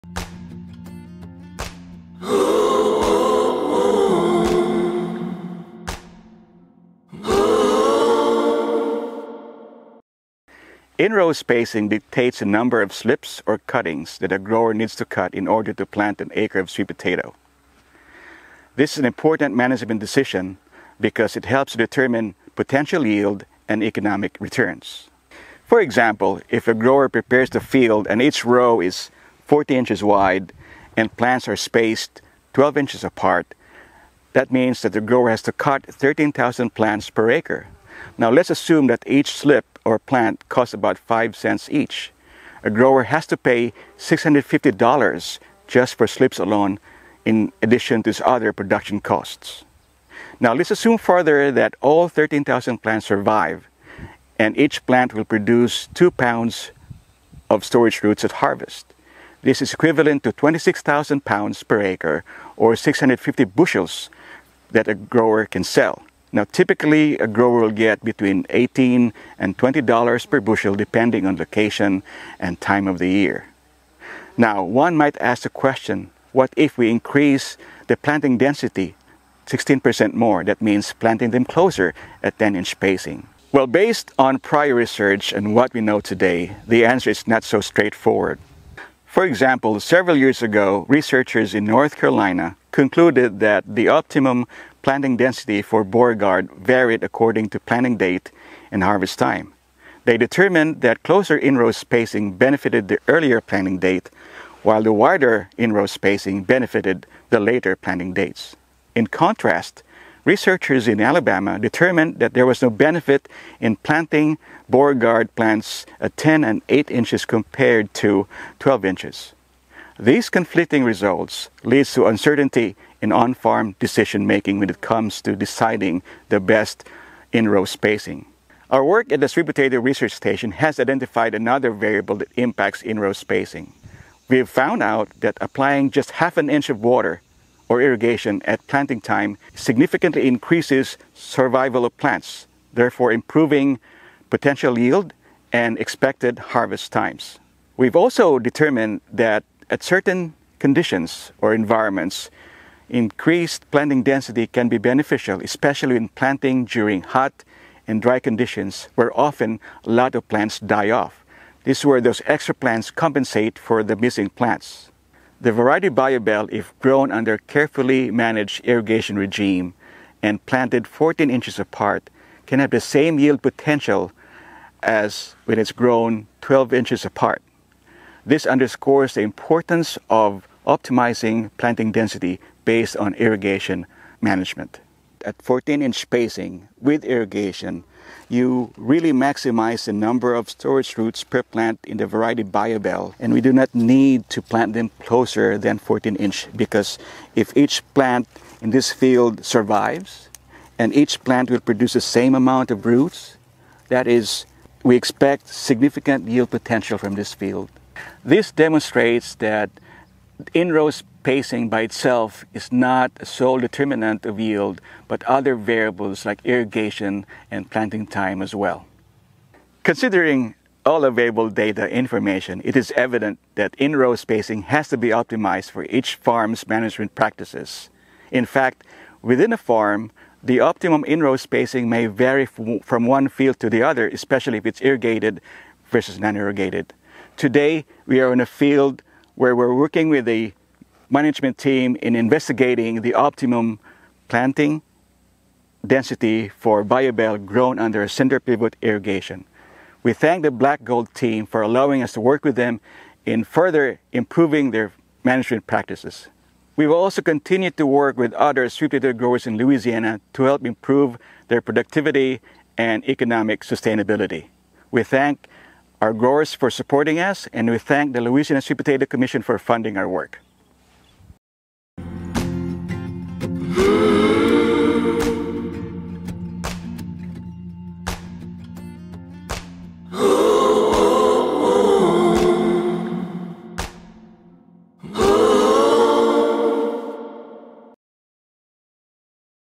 2023 Virtual Sweet Potato Field Day: Irrigation Management and Planting Density
The LSU AgCenter recently held its 2023 Sweet Potato Field Day August 31. These videos feature some of the most recent sweet potato research, as well as industry updates. Some of the presentations explored:

— Advanced sweet potato breeding lines.

— On-farm validation of irrigation management and planting density for optimized harvest scheduling and yields.

— Nematicide options and application methods.

— Pretransplant weed management with the Optogen herbicide.

— Updates on the CleanSEED program.

— Industry and crop updates